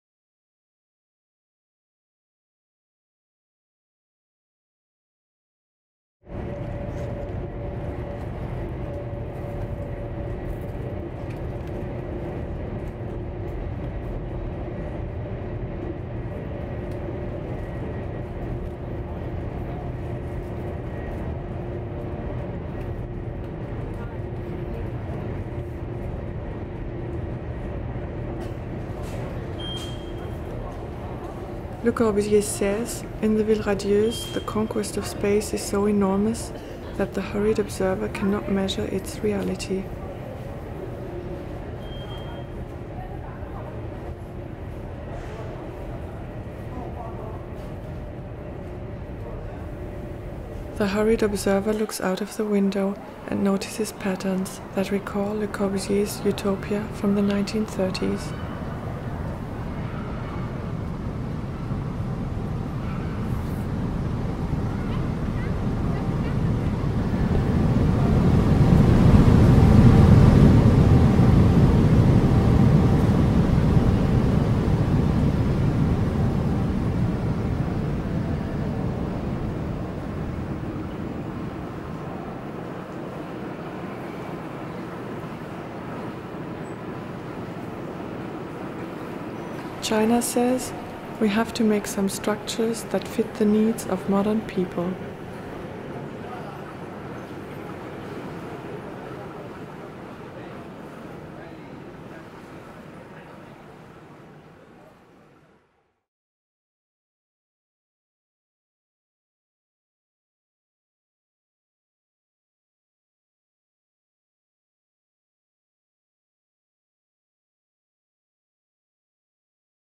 STADT/BILD. Welcome to the Jungle - Trailer von/by Ute Adamczewski
KW Institute for Contemporary Art
WELCOME TO THE JUNGLE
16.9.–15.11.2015
Group exhibition
Curated by Ellen Blumenstein
with a curatorial contribution by
Filipa Ramos